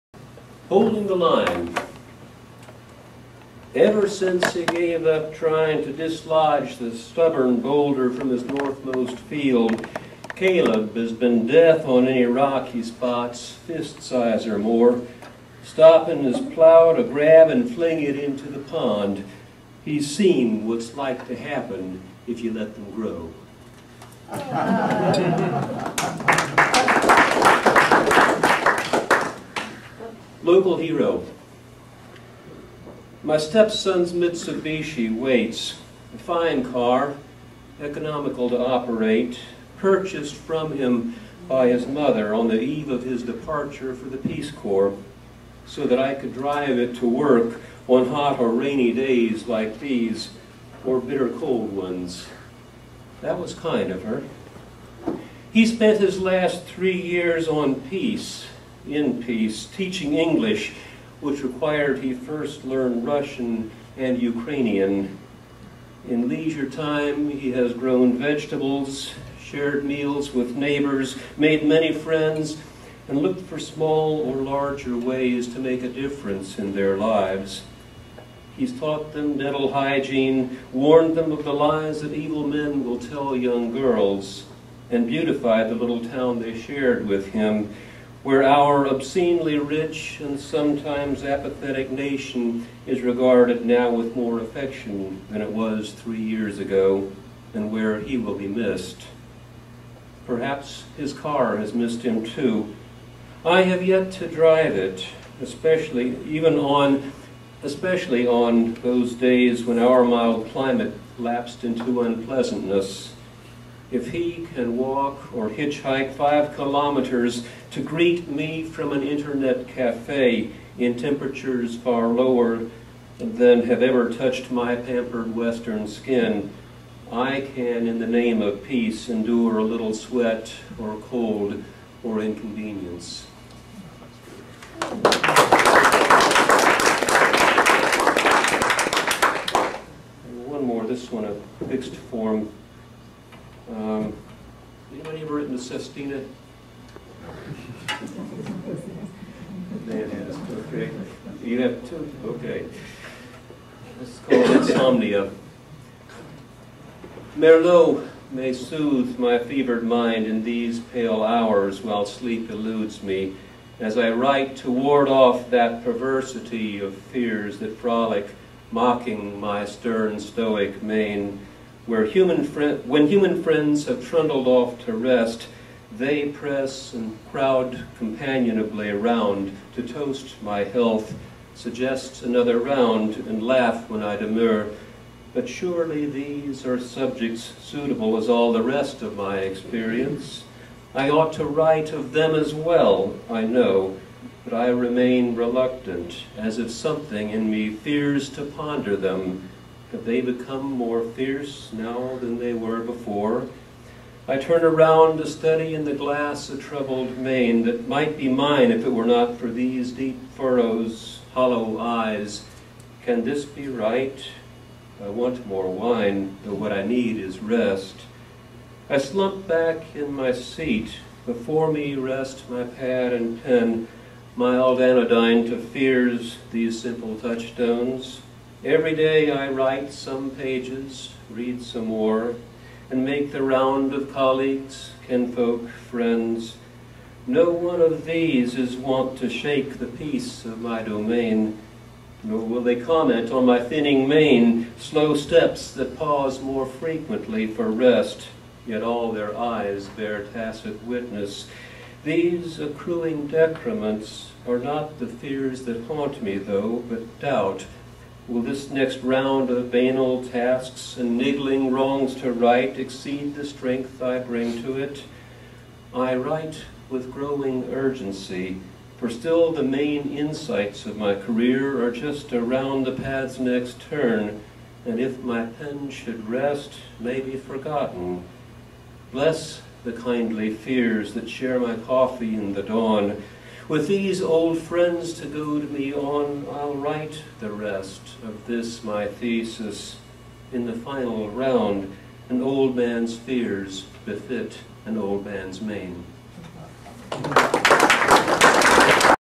Ed Robson
Winston Salem Writers Open Mic at the Community Arts Cafe in beautiful downtown Winston Salem